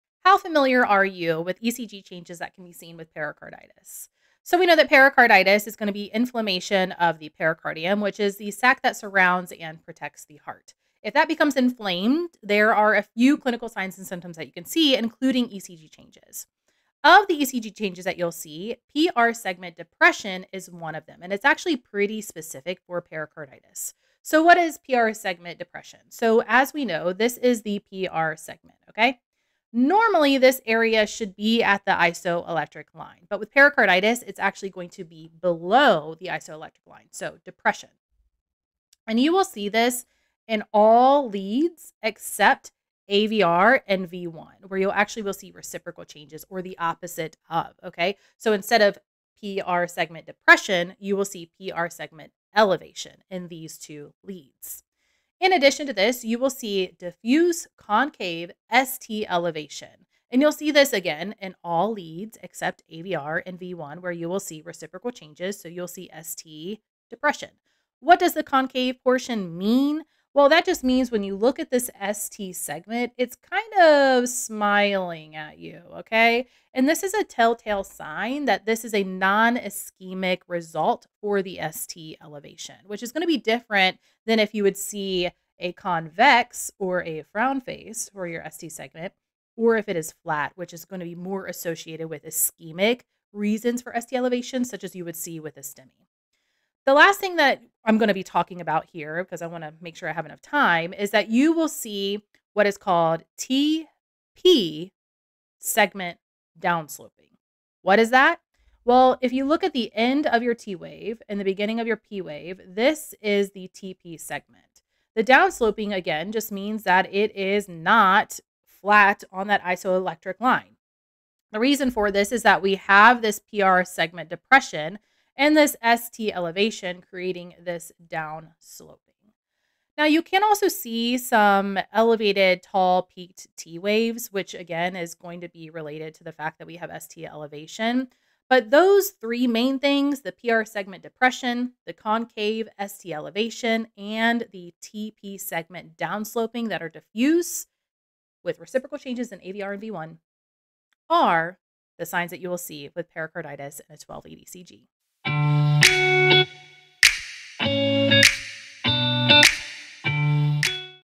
Pericarditis ECG Changes Explained | ICU Nursing & CCRN Exam Study Tip
Recognizing pericarditis on an ECG is a concept  ICU nurses must know and is often tested on the CCRN exam. This video breaks down the classic changes and how they differ from other cardiac conditions.

🎯 Perfect for:
ICU nurses • CVICU Nurses • ER nurses • Critical Care Nurses • New ICU nurses • New Grad ICU Nurses • CCRN Exam Prep